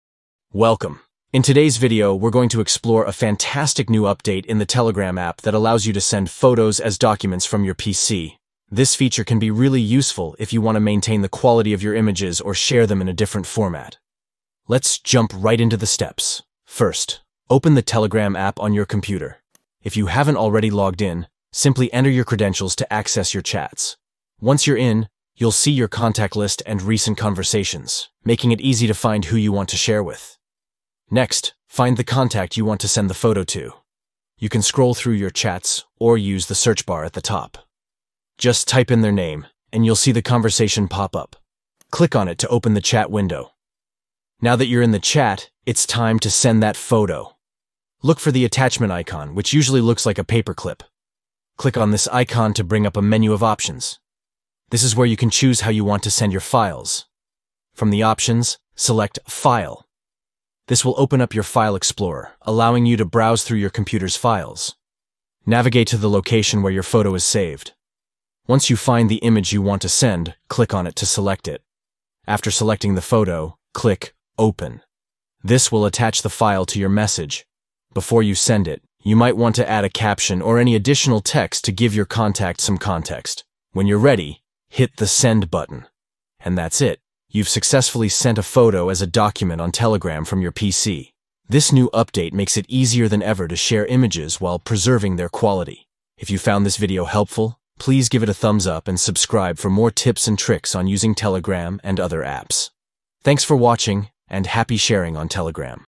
How To Send Photo In Document In Telegram App On PC (New Update)
Learn how to send a photo as a document in the Telegram app on your PC with this straightforward guide. Whether you're sharing important images or simply sending a favorite picture, these easy steps will help you do it quickly and effectively.
Steps:

Open Telegram App
Find the contact you want.
Click on the attachment icon.
Choose File, choose the photo.
Click Open, then Send.

Chapters:
00:00 Intro
00:12 Soultion
01:05 Outro